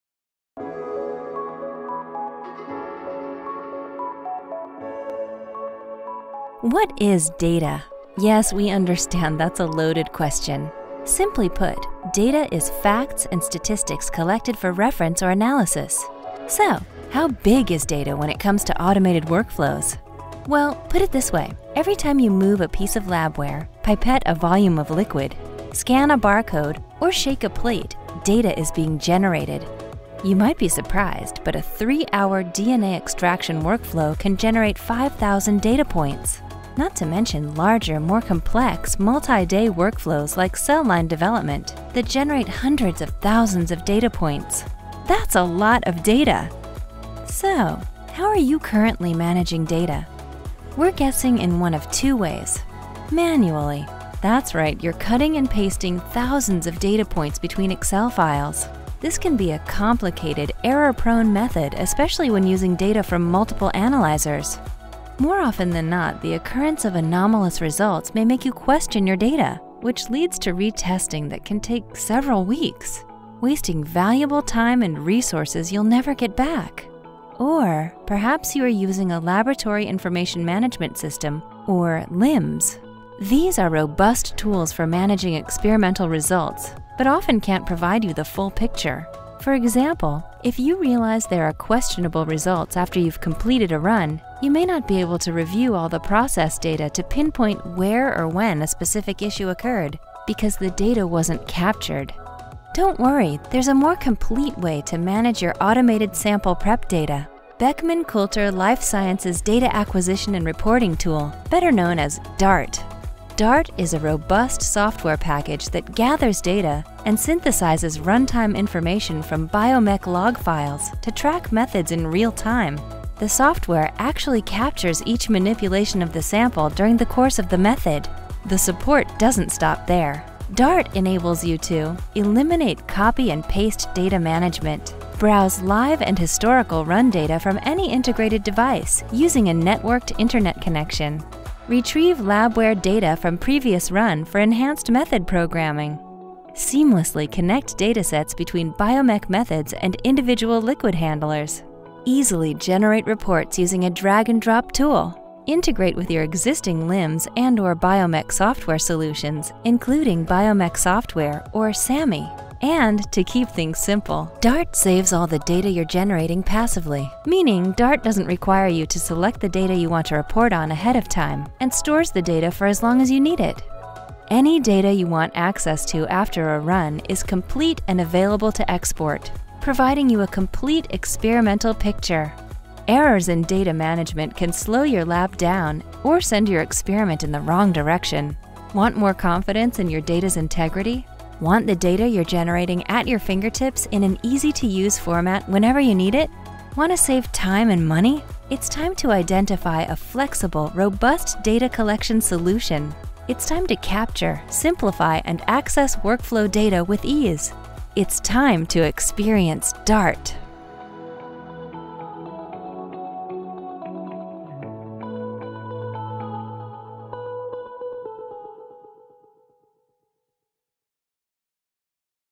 DART (Data Acquisition and Reporting Tool) Software
DART (Data Acquisition and Reporting Tool) is a software package that gathers data and synthesizes run-time information from Biomek log files to capture each manipulation of the sample during the course of the method.